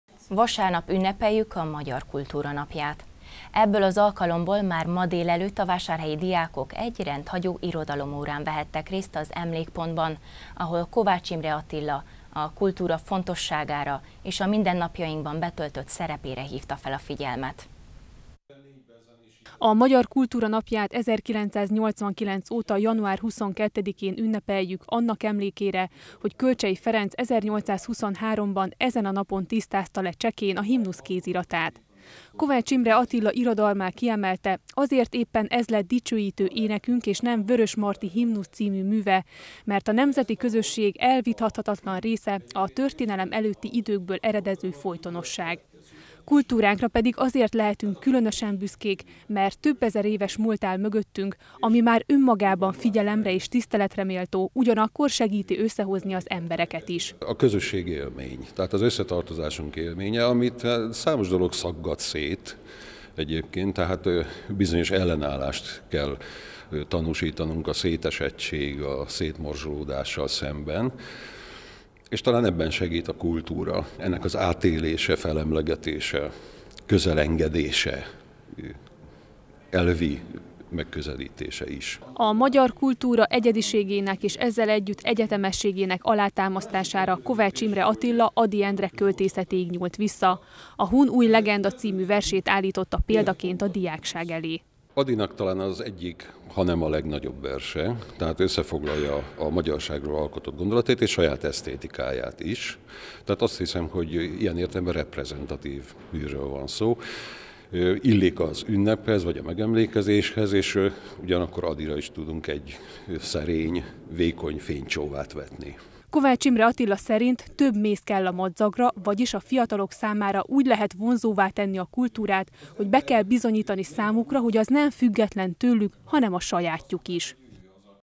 Rendhagyó történelemóra a Magyar Kultúra Napja alkalmából
Vasárnap ünnepeljük a Magyar Kultúra Napját. Ebből az alkalmamból már ma délelőtt a vásárhelyi diákok egy rendhagyó irodalomórán vehettek részt az Emlékpontban, ahol Kovács Imre Attila a kultúra fontosságára és mindennapjainkban betöltött szerepére hívta fel a figyelmet.